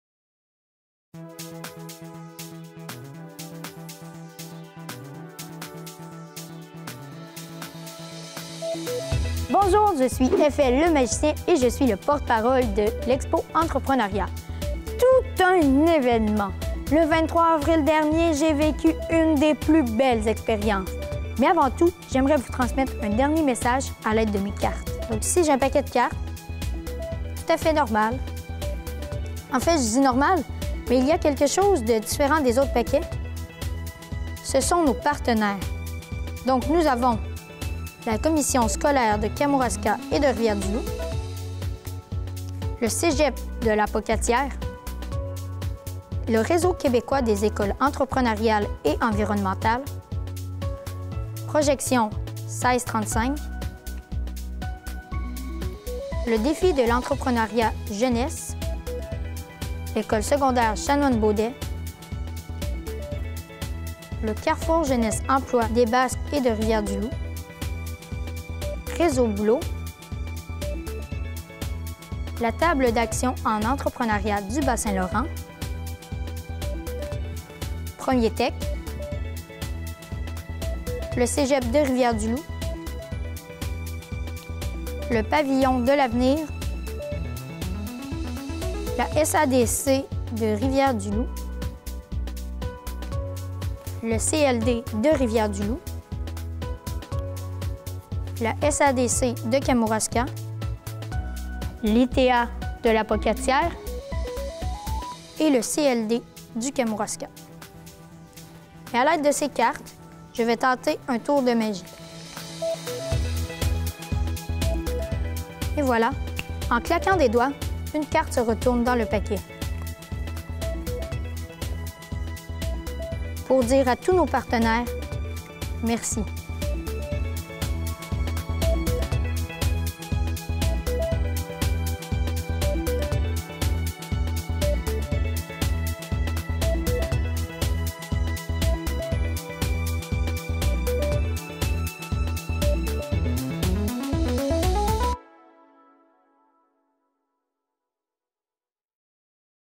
Expo-Entrepreneuriat 2015 | Capsule #4
Mot de la fin de François-Luc et remerciements à tous les partenaires.